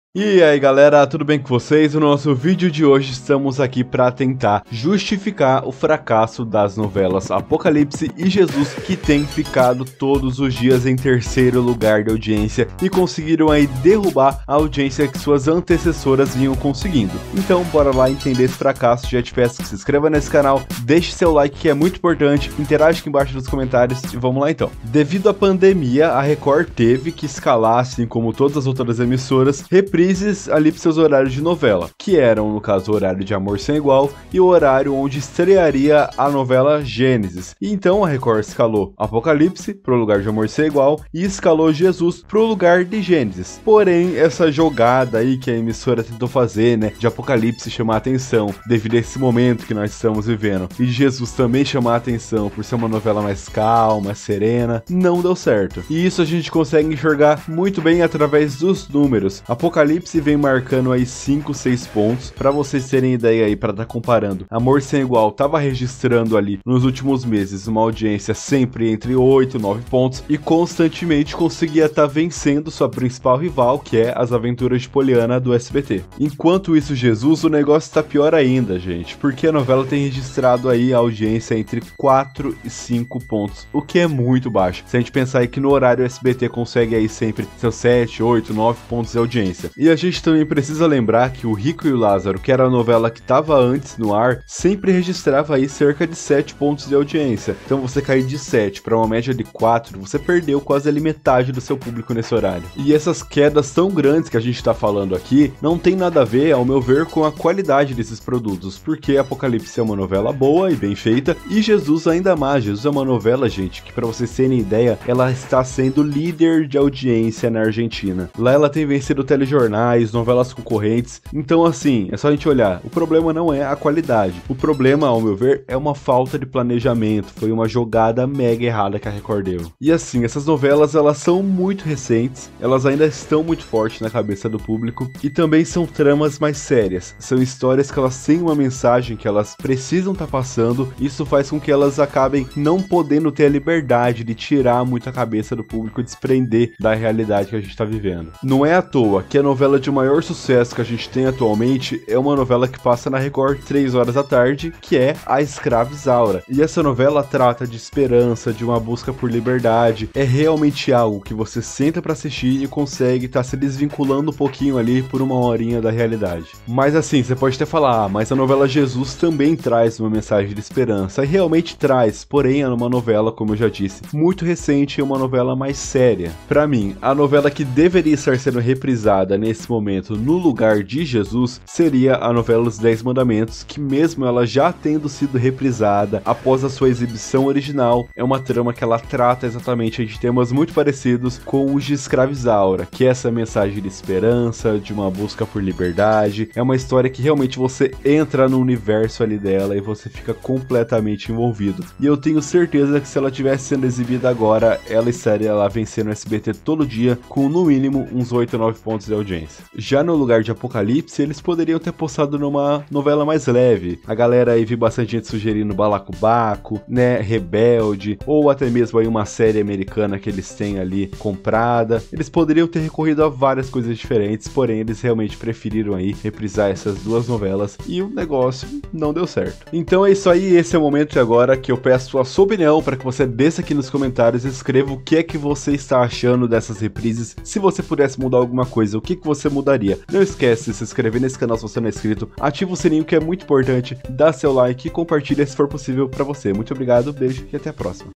NOVELAS APOCALIPSE E JESUS FAZEM AUDIÊNCIA DA RECORD DESPENCAR! POR QUE?
A audiência das novelas "Jesus" e "Apocalipse" tem sido cada vez mais preocupantes. Por mais que ambas novelas possuam uma qualidade invejável, elas não tem conseguido chamar a atenção do público e nesse vídeo busco entender os motivos disso. Amor Sem Igual volta em breve e pode encontrar um cenário preocupante nas novelas da Record TV.